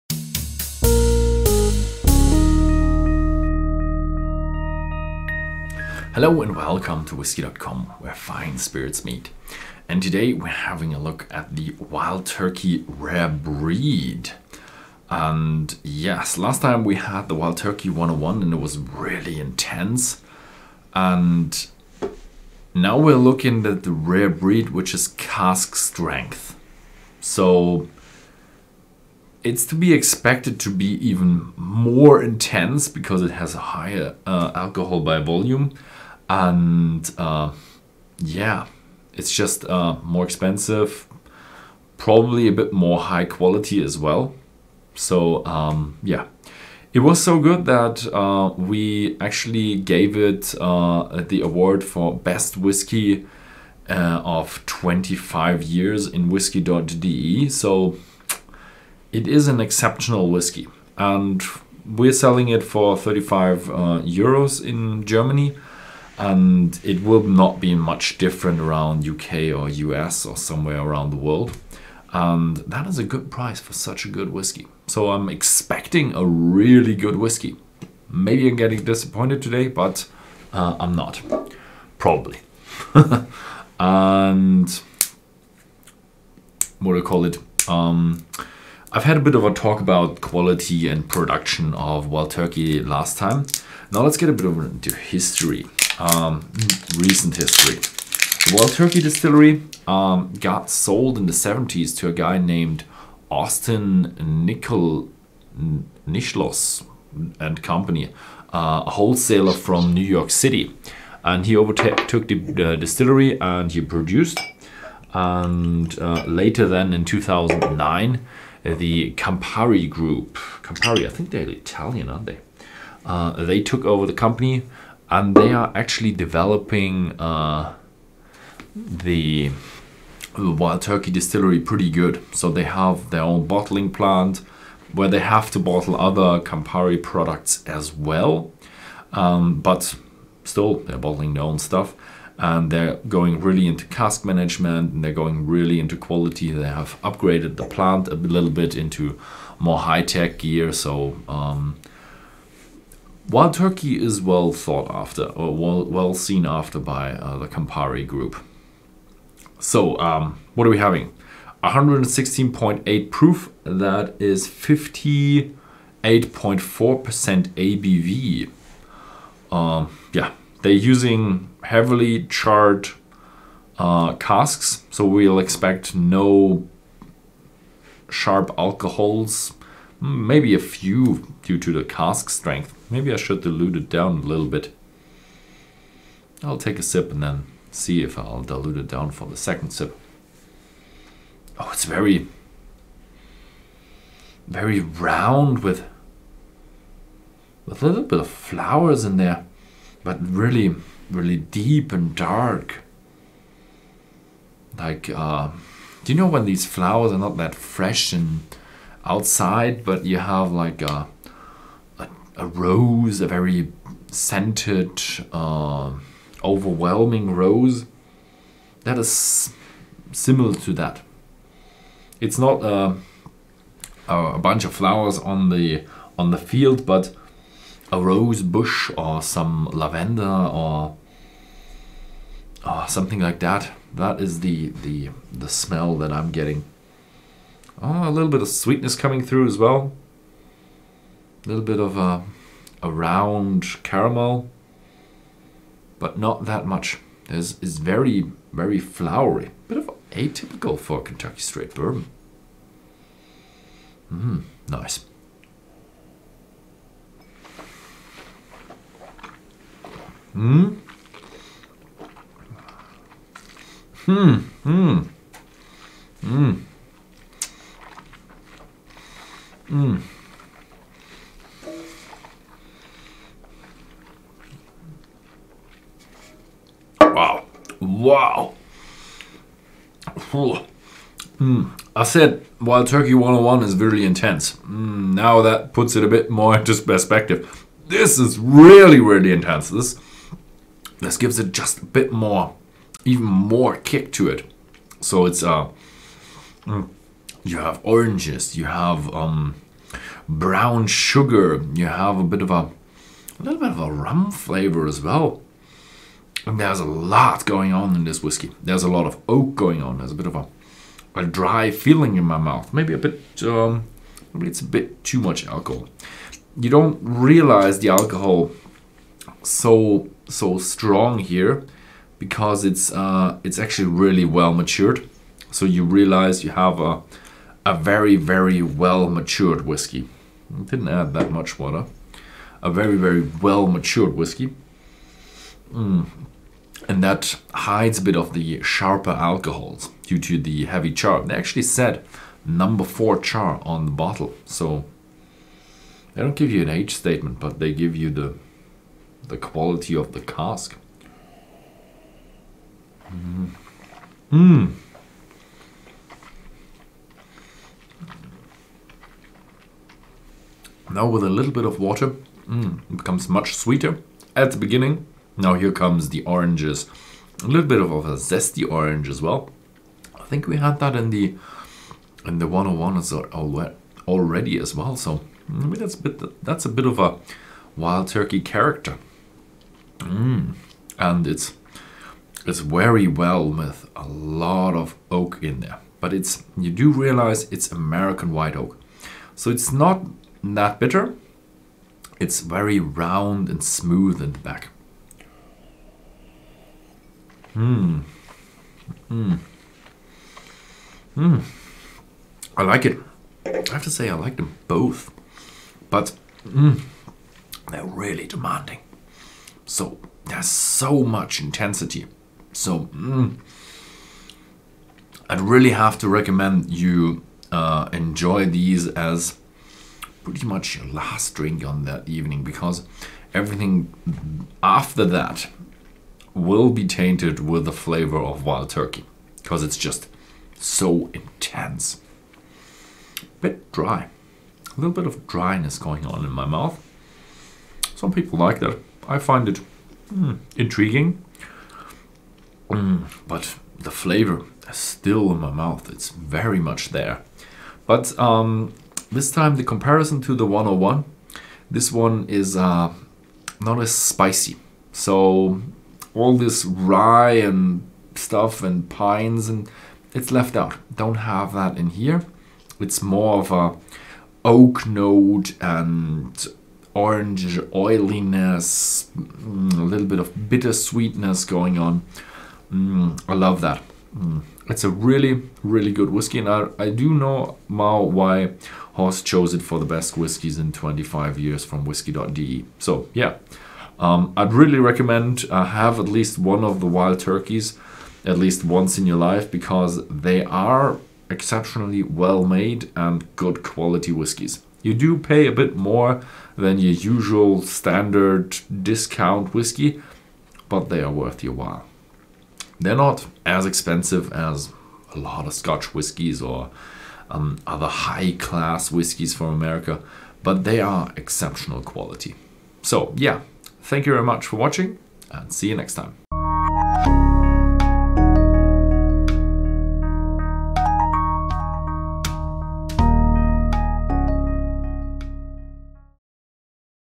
Wild Turkey Rare Breed
Nosing 3:00
Whisky.com reviews the Wild Turkey Rare Breed.